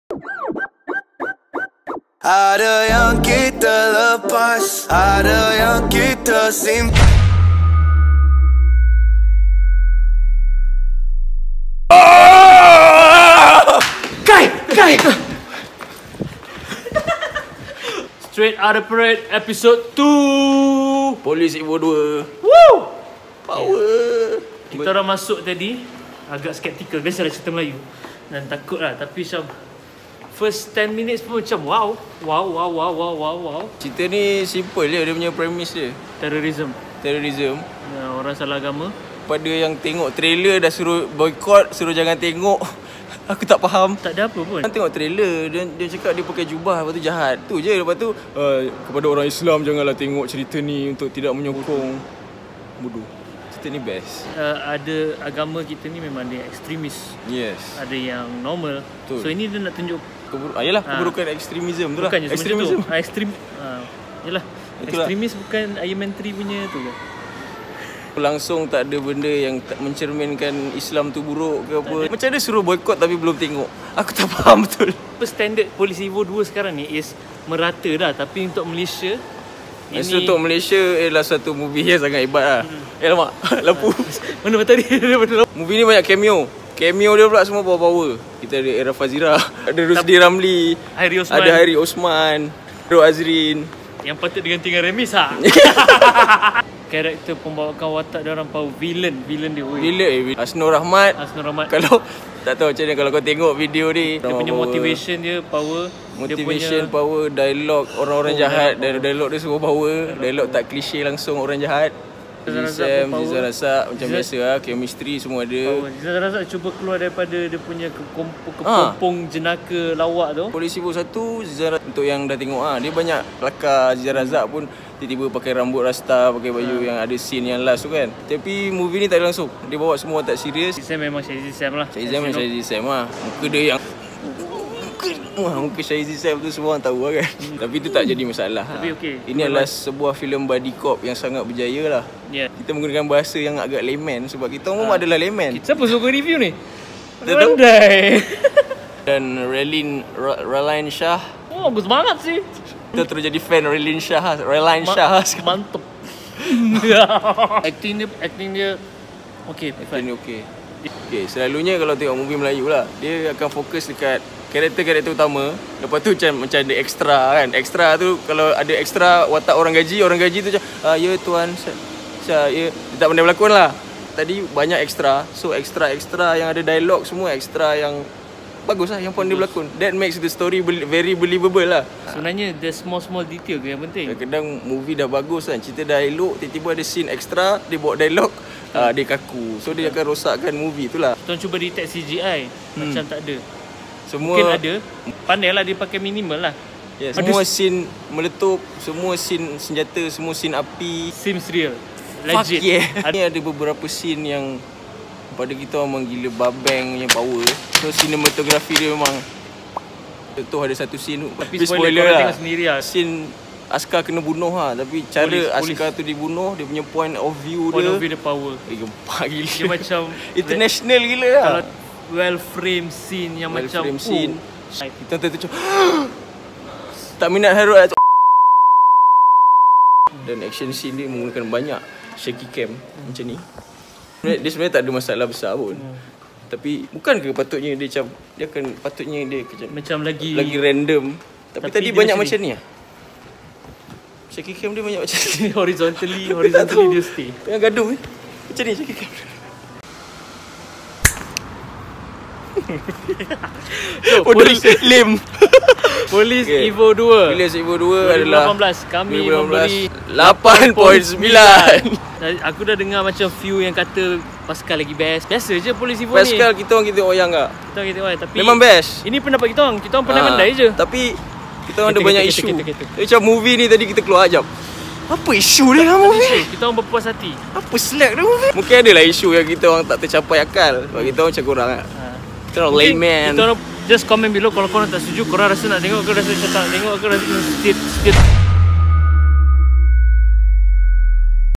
POLICE EVO 2 (2018) - MOVIE REVIEW // STRAIGHT OUTTA PARADE - EP 02
Welcome to "Straight Outta Parade" , a raw unfiltered series, where we review movies straight away after coming out of the cinemas! Only at Wayangpeople!